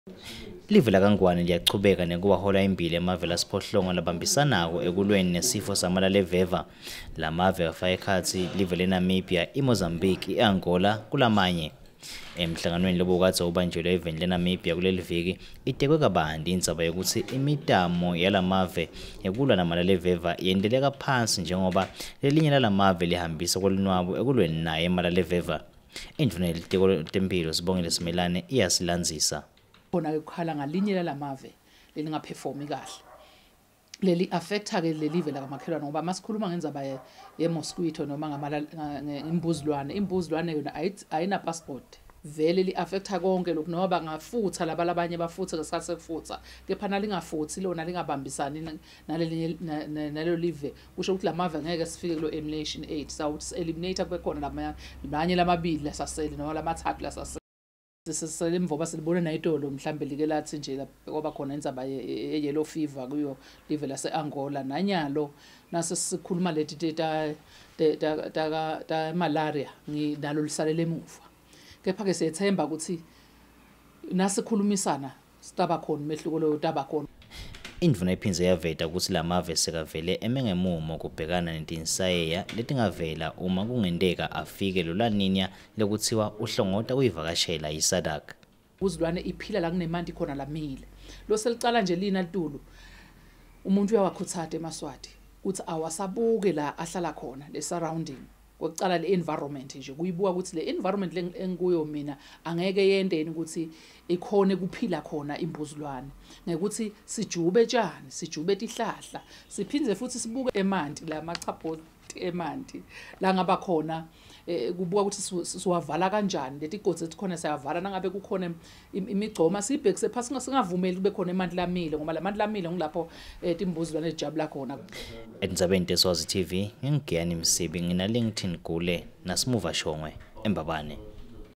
SADC Fighting Malaria
Eight countries under the SADC who are working together in fighting Malaria are concerned about the lack of interest from Angola in joining the fight against Malaria. This was revealed by The Minister of Health Sibongile Simelane who is the Chairman of the eight countries.